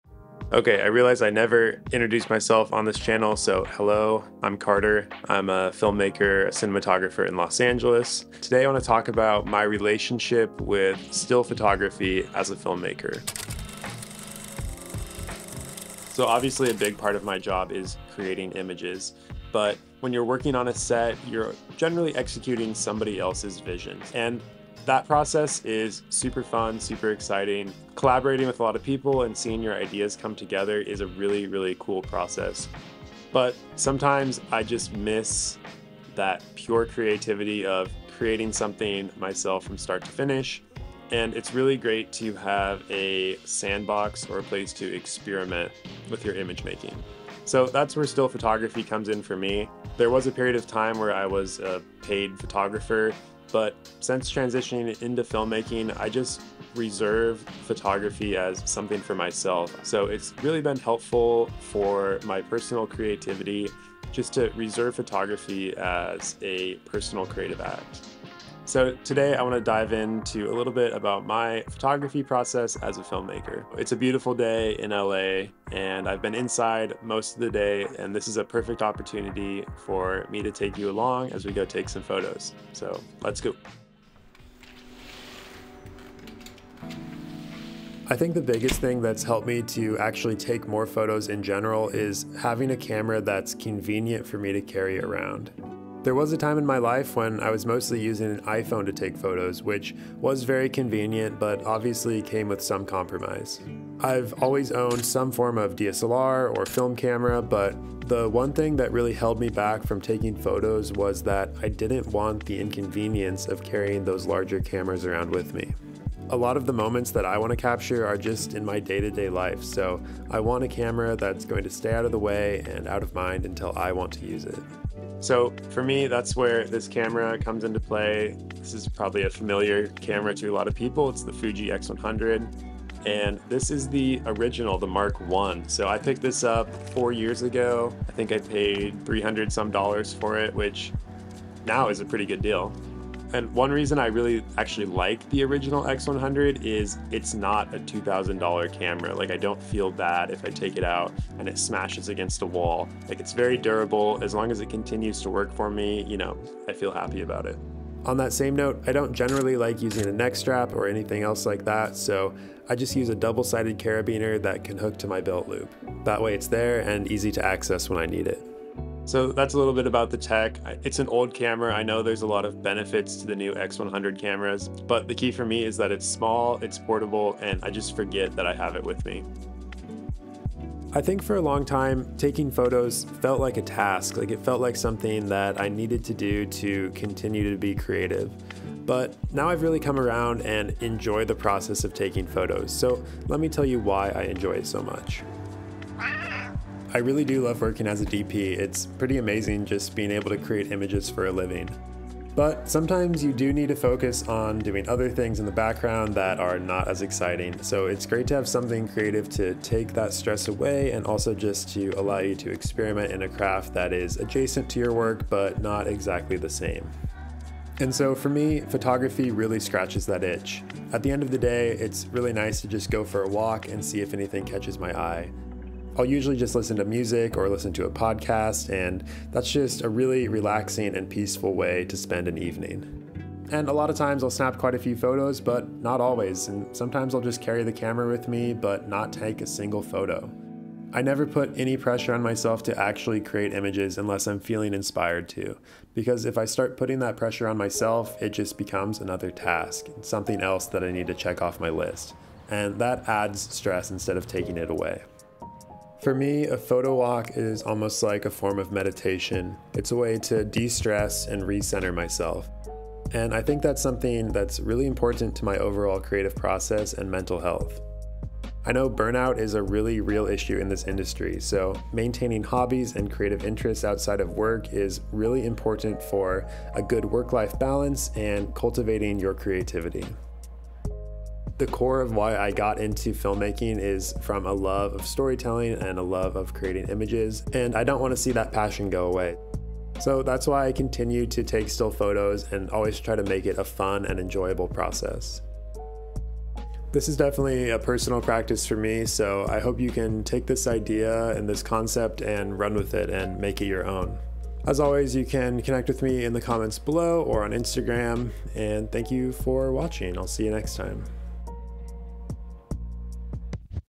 Photography for Filmmakers
My relationship with photography as a filmmaker is precious to me, something I try to keep safely guarded.

What creative hobbies do you hold outside of filmmaking? Let me know if the comments below